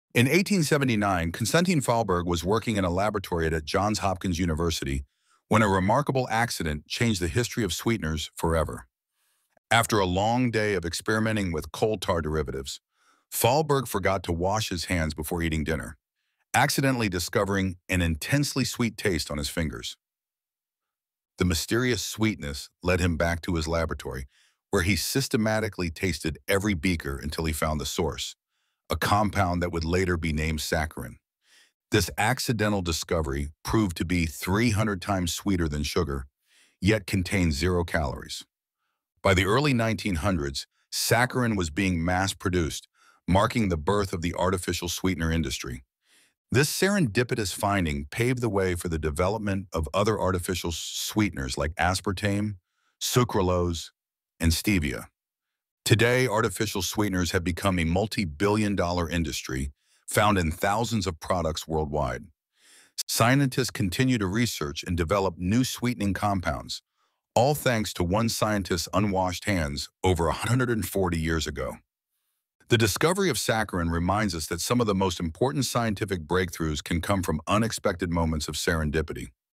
The Accidental Sweetener: How Saccharin Changed the Sugar Game 🍬
Join us on a journey back to 1879, where a simple mistake in a lab led to the discovery of saccharin - a game-changing artificial sweetener! Discover how one scientist's unwashed hands transformed the sweetener industry forever. From vintage labs to modern supermarkets, witness the evolution of sweeteners and the serendipity behind major scientific breakthroughs. 📚✨